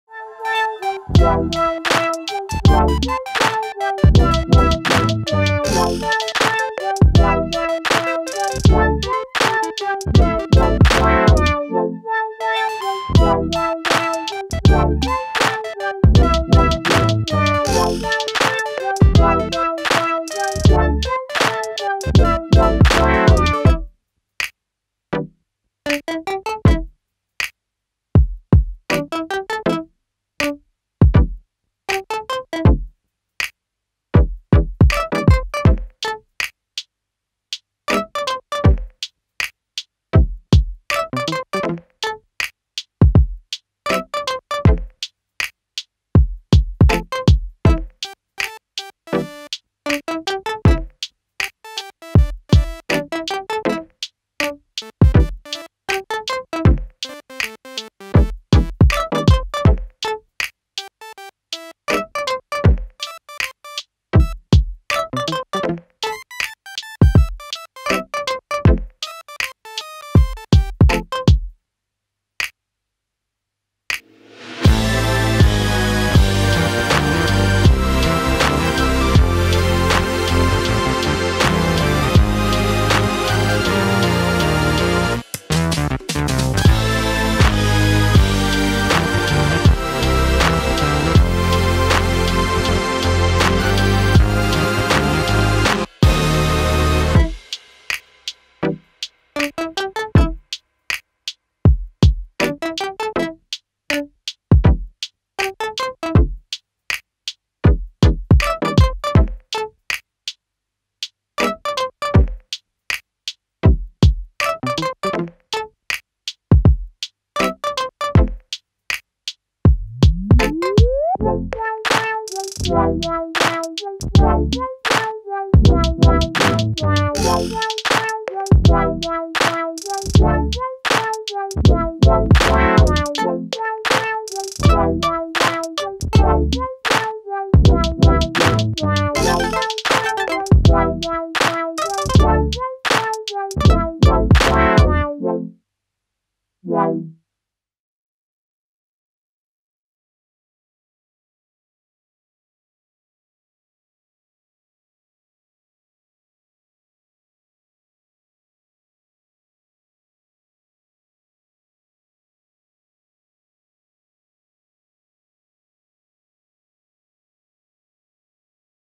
Sandy Lookbook | Fall Neutrals
Sandy beigey tones like tan and camel are everywhere this season so I decided to style a few looks in these neutral tones! I’ve styled a dress, pants, an off-shoulder top, and even a jumpsuit woo.

I tried to keep it simple with the looks, editing, and number of colors. It was hard for me since I love the rainbow but it was a fun challenge to stay in this warm color scheme.

xoxo hoshiko

Video shot by hoshiko
Edit by hoshiko